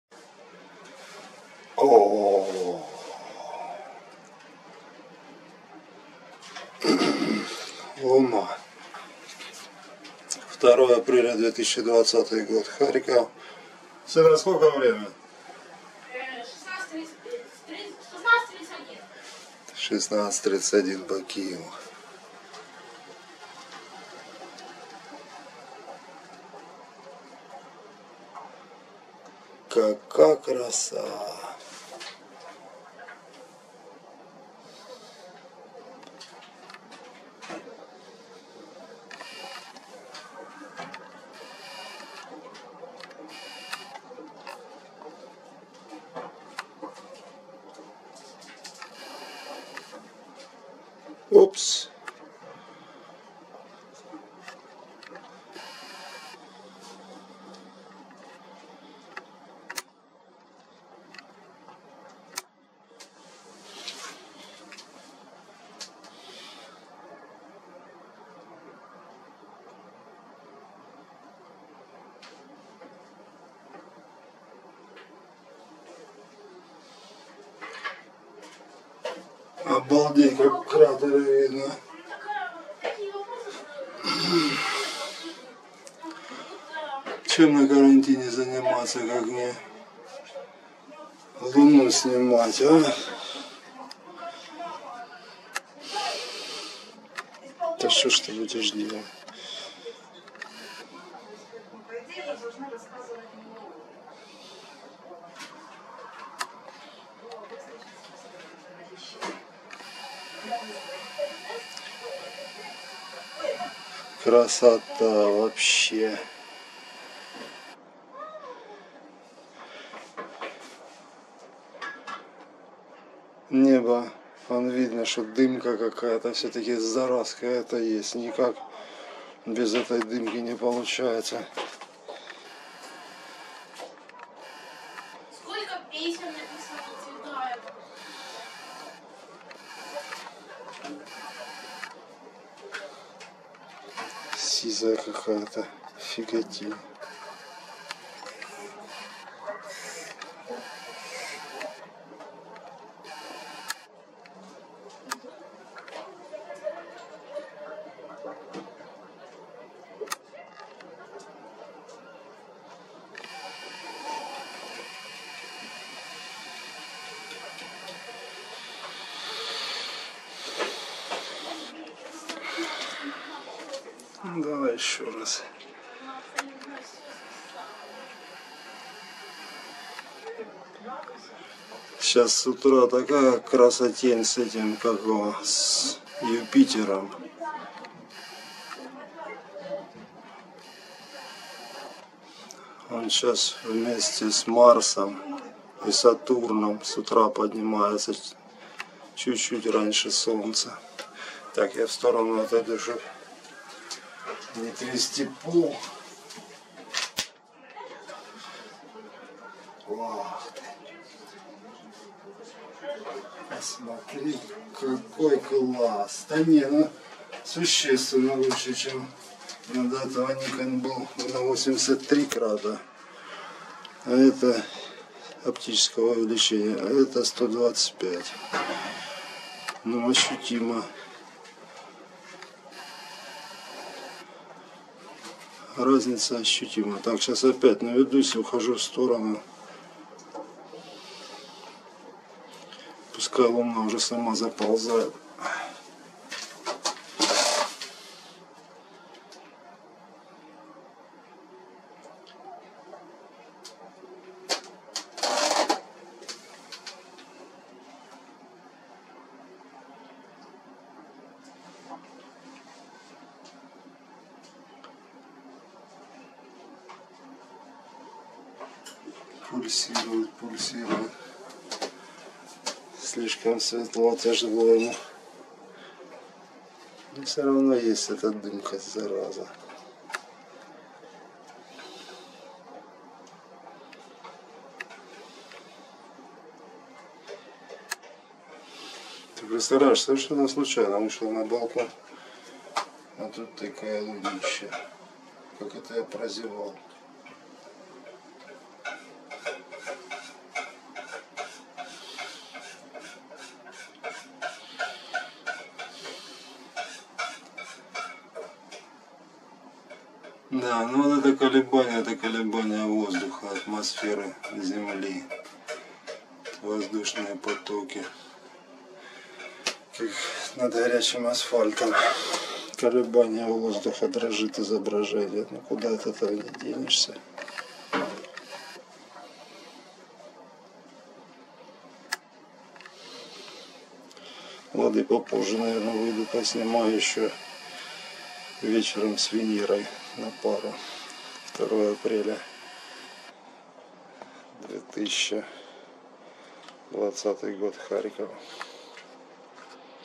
Луна в 125-кратном увеличении 2 апреля 2020 год Харьков
Луна над Харьковом второго апреля двадцатого года - снято на сто двадцати пятикратный  оптический зум фотоаппарата Nikon Coolpix P1000.